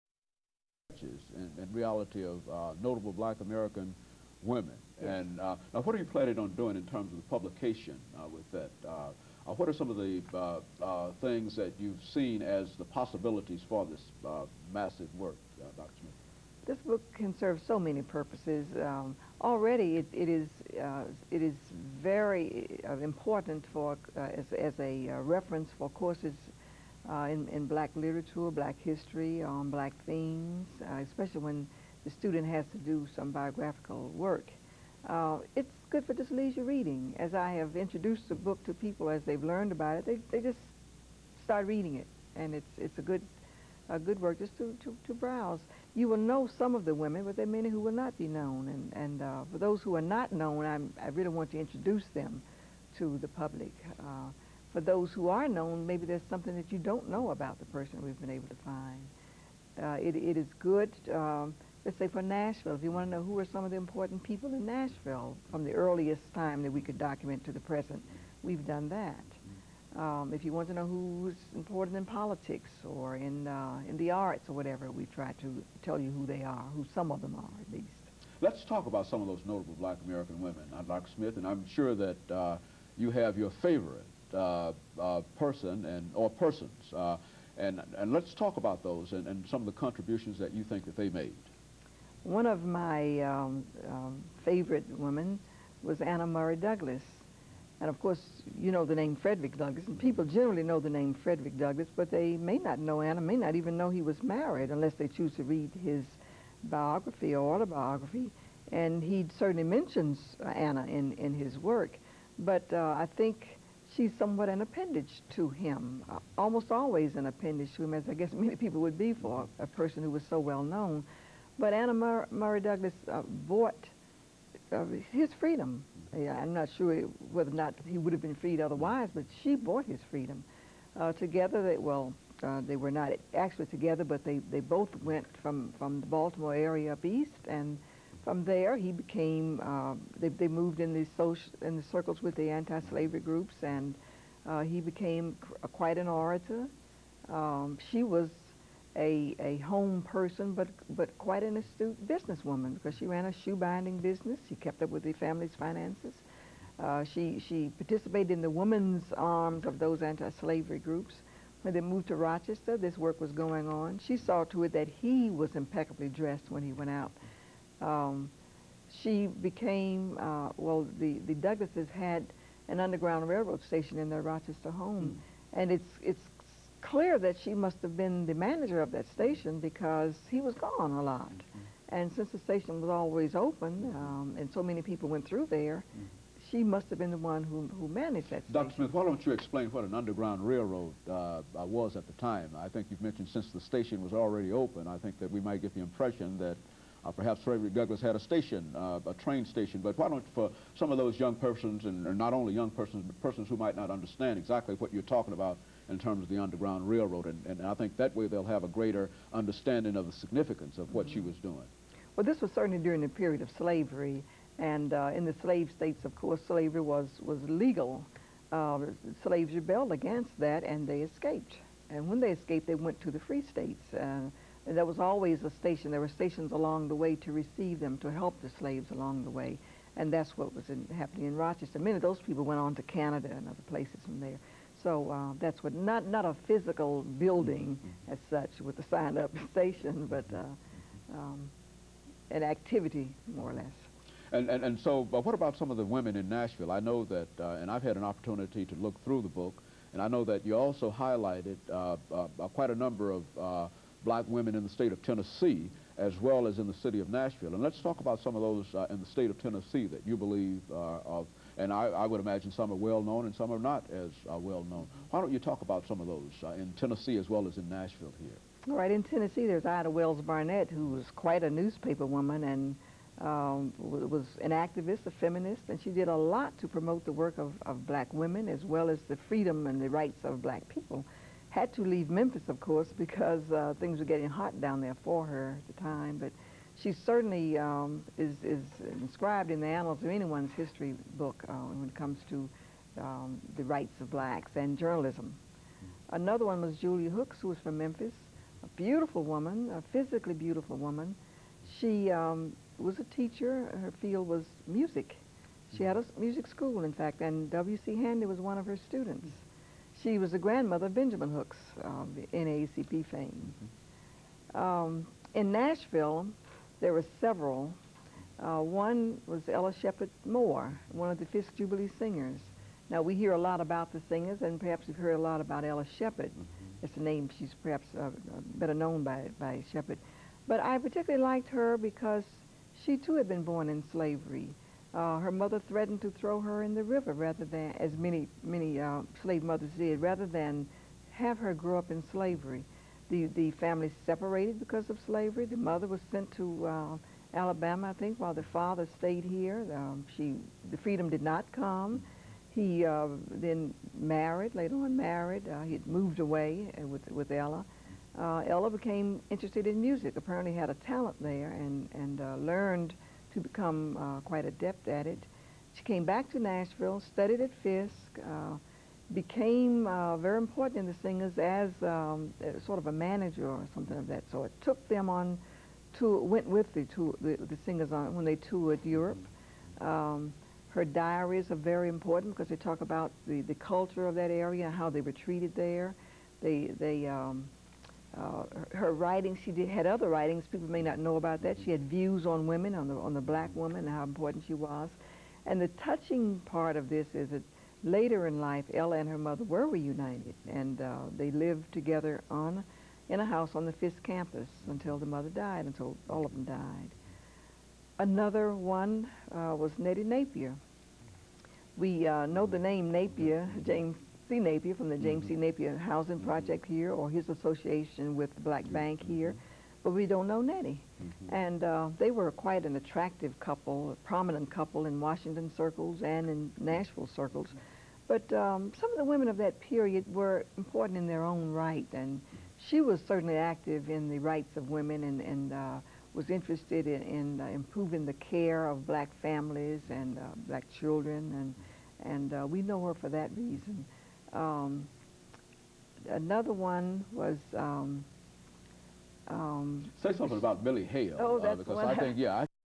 Black American Females, JSmith2
Comments With Dr. James Haney Presents*Black American Females, with JSmith, Librarian, Fisk University, Nashville, Tennessee, Part 2 of 4